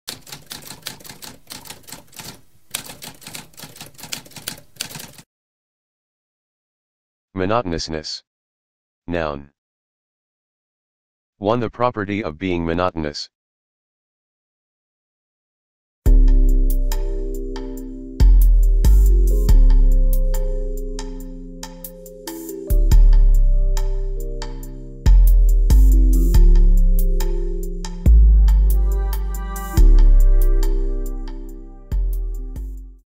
What does monotonousness mean?
A spoken definition of monotonousness.

Intro Sound:
Typewriter - Tamskp